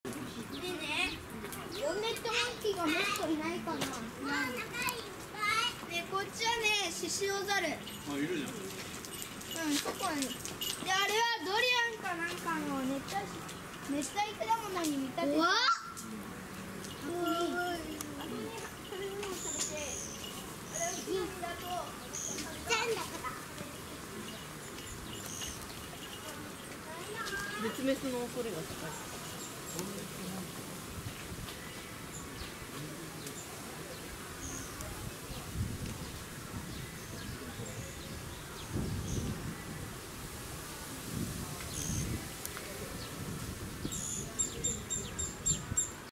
シシオザル (ときわ動物園) 2018年5月19日
シシオザル (ときわ動物園)

□ シシオザル
英名：Lion-tailed macaque
学名：Macaca silenus

■ 撮影場所
ときわ動物園 (山口県)
Tokiwa Zoo